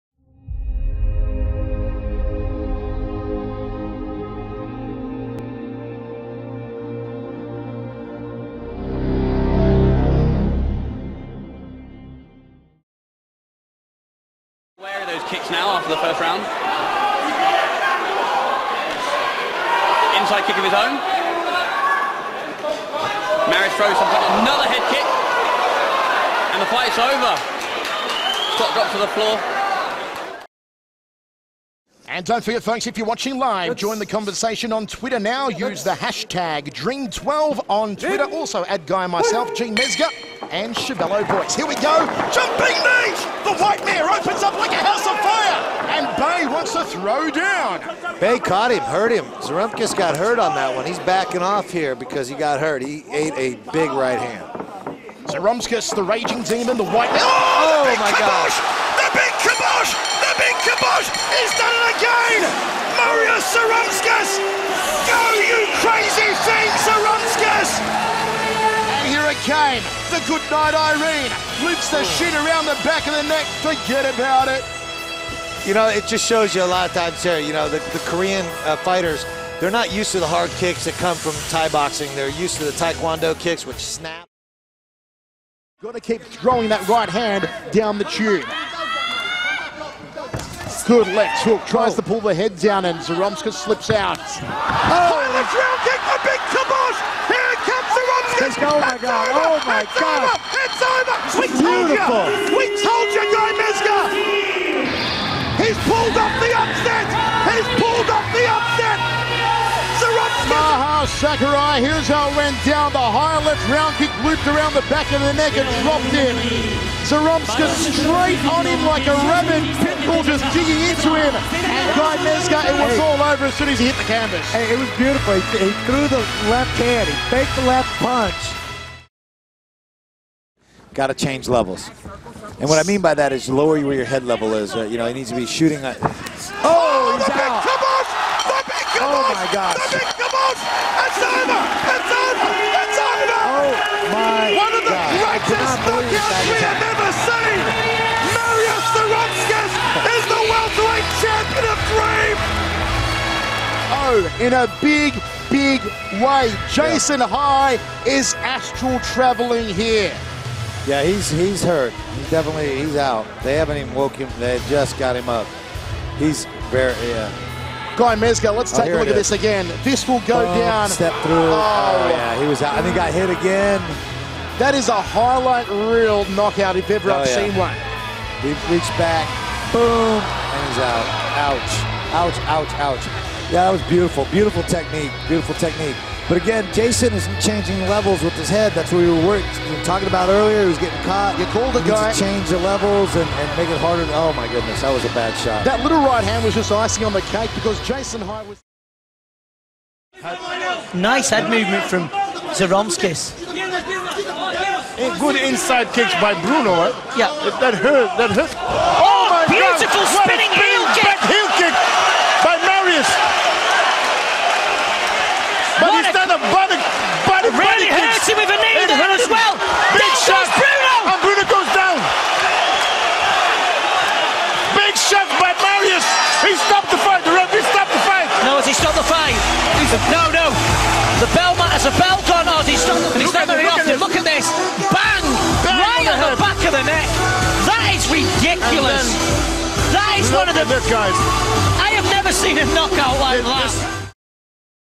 Marius Zaromskis - Top 5 Knockouts
Continuing the top 5 finishes -series, this time with Marius Zaromskis, fighter that I really like and fighter that I find to be underrated in terms of entertainment, just very fun fighter to watch with devastating kicking game